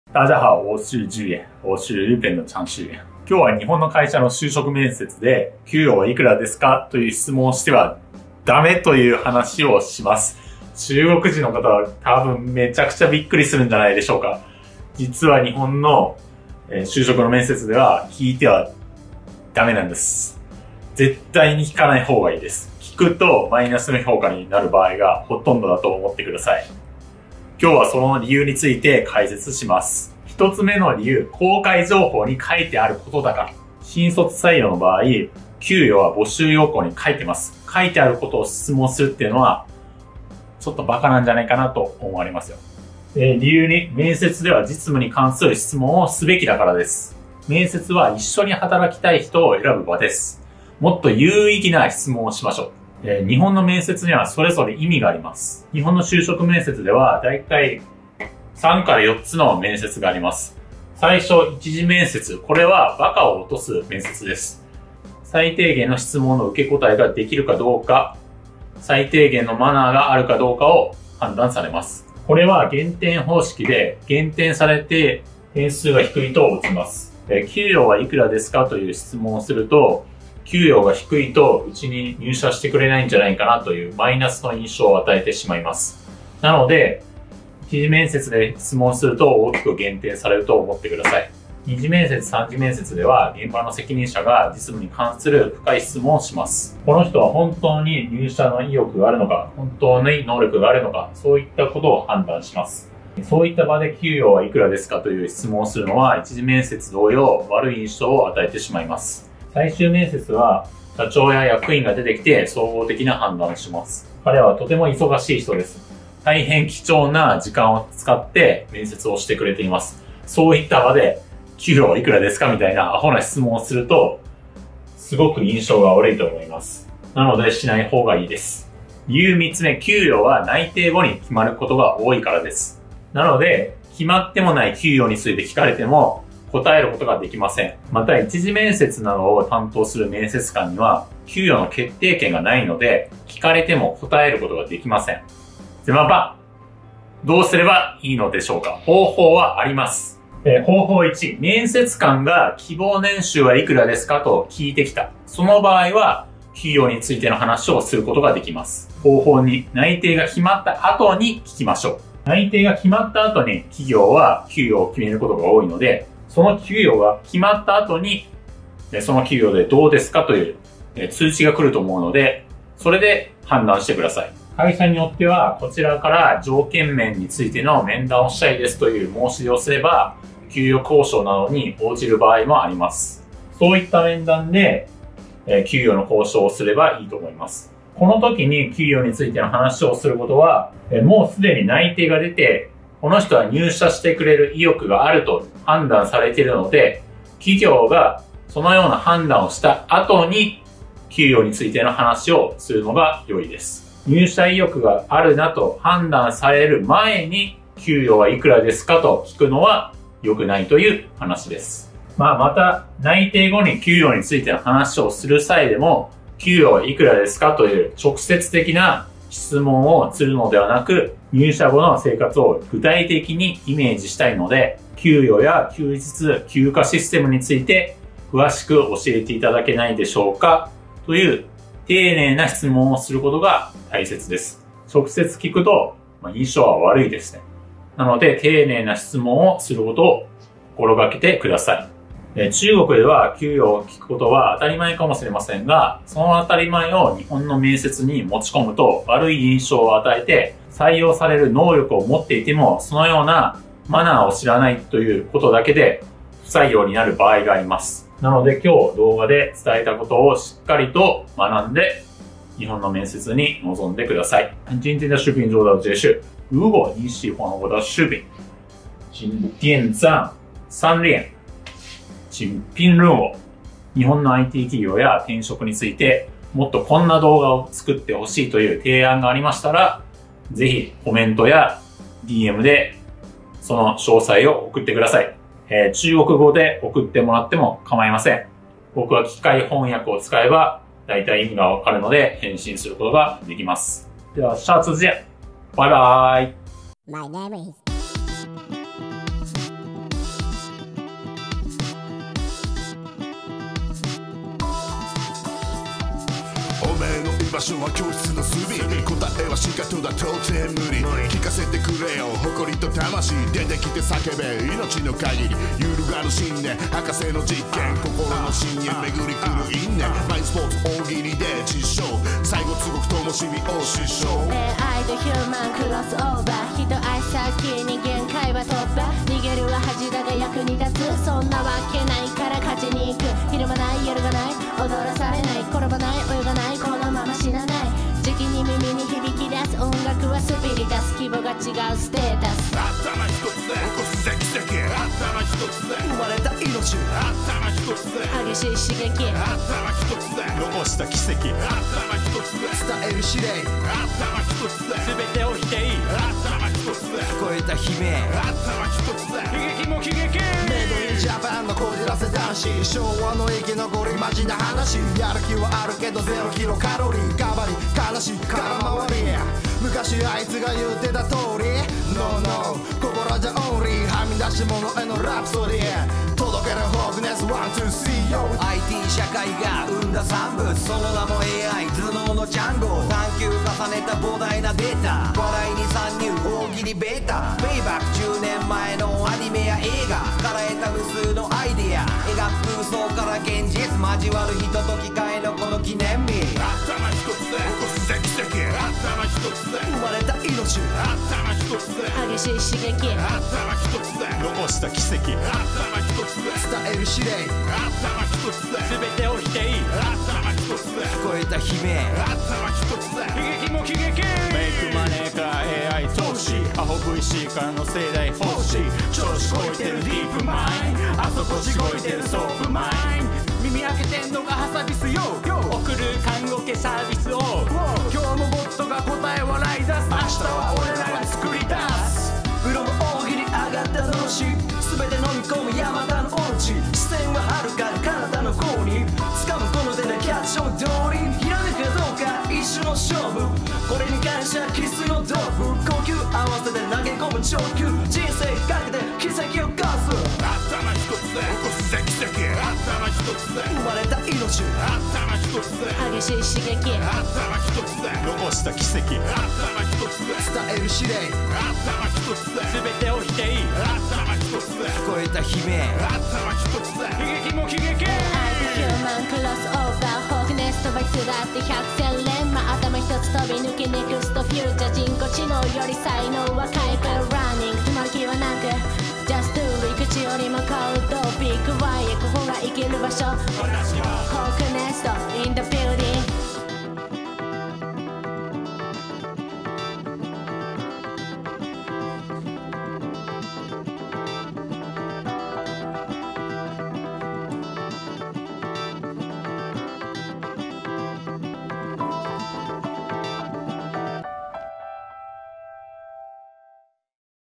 日本人告诉你为什么在日企就职面试的时候不能问‘工资有多少’【日语口语】【日语听力】【中文字幕】
@我是直也 哔哩哔哩(Bilibili)

日本人。中日友好交流活动。正在学中文。在东京。程序员。

今天的话题是在日企就职面试时不能问工资有多少
中国的各位应该会吓一跳对吧。实际上在日本就职面试的场合是不允许提问的
最好尽量不要去提问。一旦问了的话，大部分情况都是得到负面评价。
今天就来解说一下这个理由。其一就是在公开信息上已经注明了。应届毕业生录用时，工资就写在了招募要点上。已经写清楚了再去提问
不就成了明知故问了对吧。其二就是，在面试时应当问一些有关实际业务的问题。
面试就是在选想一起共事的人。多提问一些有意义的问题吧。日本的面试都有各自的意义。日本的就职面试大概有3-4个流程。初次面试就是一个淘汰傻瓜的环节。
能否应答最起码的问题，判断是否具备最基本的礼节。这就是采用扣分方式来淘汰分数低的人
如果主动问工资有多少的话，会给人一种工资低了就不愿进公司的负面印象。试想一下初次面试就给扣了很多分
二次面试、三次面试时现场的负责人会就实际业务进行深度地质询。这个人是否有入职意愿，是否真的有具备能力、
就是按照面试者的回答来判断的
在回答时询问工资有多少也会跟初次面试一样给人不好的印象
最终面试是由社长或董事出面来进行综合的判断。他们都是大忙人、因此腾出宝贵的时间来给新人面试。如果这时候问了工资是多少这样的蠢问题
印象会变得非常糟糕。因此还是不问为好
理由其三就是工资往往是在内定之后才决定的。所以即使被问到了这类问题、由于没有确定工资所以也无法回答
再者就是负责初次面试的面试官没有工资的决定权，所以被问了也回答不上来
那么该如何是好呢，还是有办法的。方法一、一旦面试官问了希望的年薪是多少，那种情况下可以谈工资
方法二、确定内定之后在打听吧。因为企业确定内定之后才决定工资的情况比较多，我想工资定下来后会有通知说那个工资怎么样，所以请根据通知来判断。根据公司的不同，如果我们提出条件方面的面谈申请的话
也会有相关的工资交涉的机会,我觉得在那种面谈中谈工资比较好
这时讨论工资的事已经内定了而且可以看出这个人有进入公司的意愿，所以在企业做出那样的判断后再谈工资比较好
在公司不知道你是否有入职意向时问工资多少是不好的
另外，即使在内定后谈论工资时也不要直接问工资是多少，而是要具体地想象进公司后的生活，所以关于工资和休息日休假系统能不能详细地告诉我这样细致的问题很重要
如果直接问了，印象会很差，所以细节问题要注意
在中国问工资也许是理所当然的事，但如果将那份理所当然带到的日本面试中，即使有被录用的能力也会给人留下不好的印象，但也有因为不知道这样的礼仪而被淘汰的情况。所以，请好好学习今天视频里传达的内容，参加日本的面试
关于日本的it企业和跳槽，如果需要制作更多这样的视频的话，请一定要通过留言和邮件将详情告诉我
用中文也没关系
我用机器翻译的话能明白大概意思，所以能回复。那么下期再见，拜拜ヾ(•ω•`)o